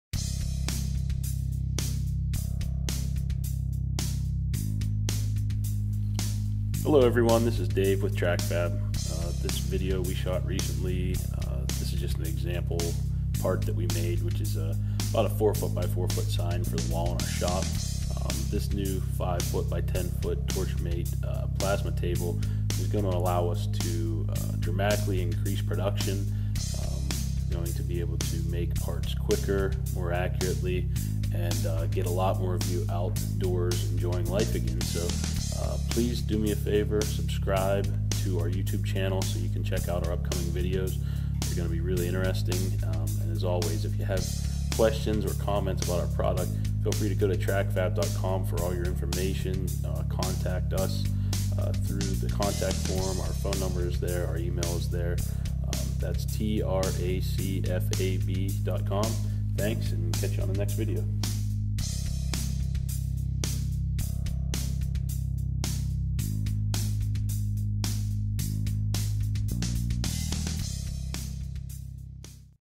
TracFab Fabrication Capabilities
This video is an example of how we can take raw metals and transform them into our products.